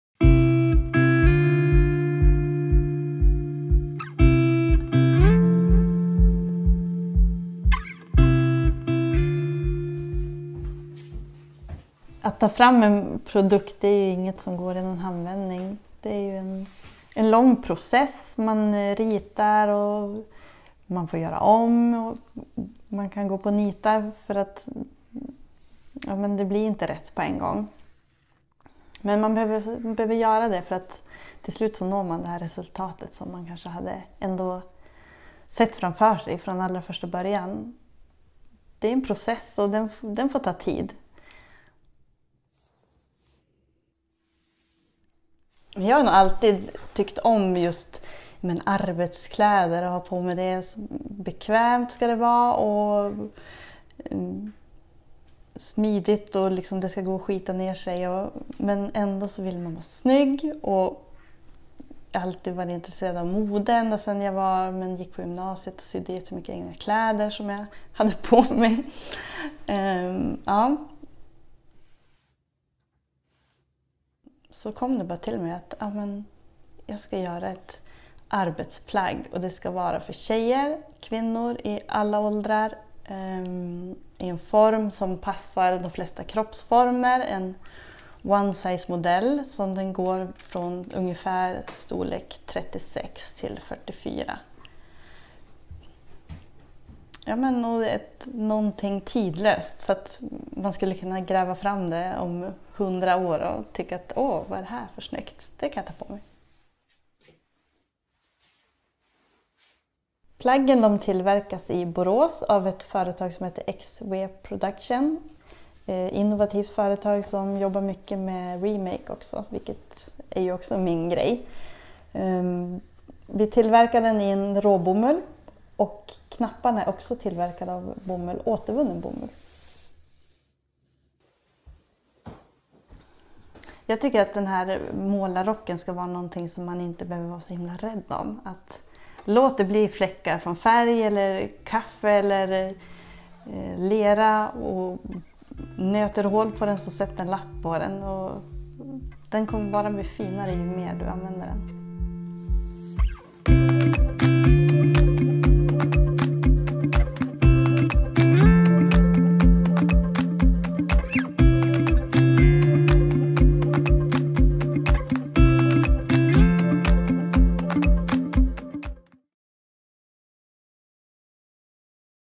eXpression Design - Ida Hansson, Nordic Remake
Under åtta månader har utvalda talanger 2019/2020 tagit en design från idé till prototyp och vidare till en färdig produkt, tillverkad lokalt, i programmet eXpression Design.

Ida Hansson, Nordic Remake, tog fram ett arbetsplagg för kvinnor.

Mer information finns på highcoastcreative.se.